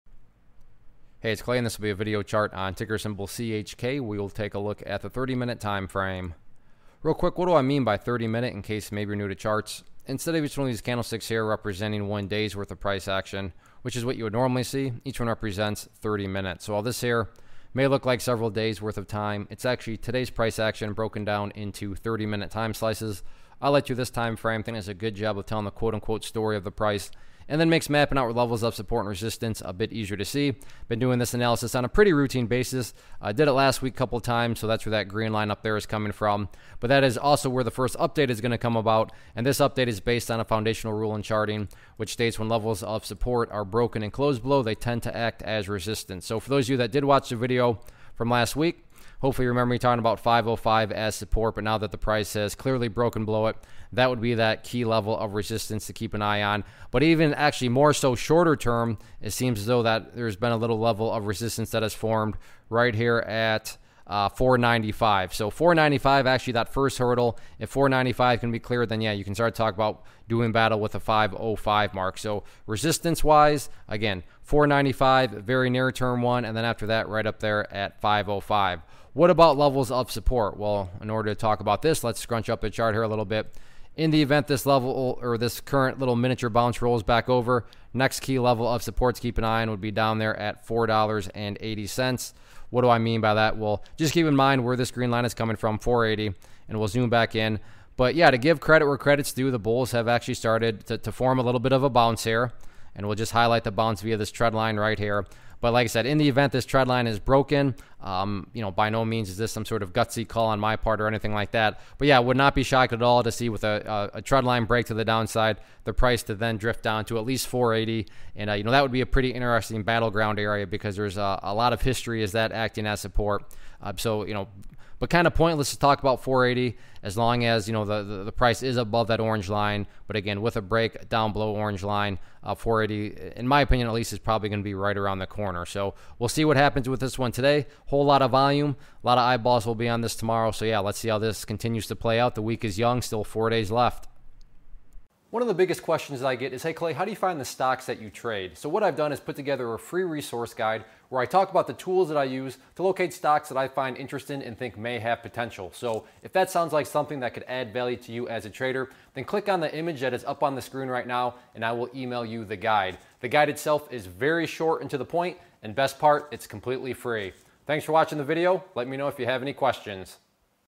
Chesapeake Energy Corporation - CHK Stock Chart Technical Analysis for 07-16-18
Learn how to read stock charts and identify technical patterns as ClayTrader does a quick stock chart review on Chesapeake Energy Corporation (CHK).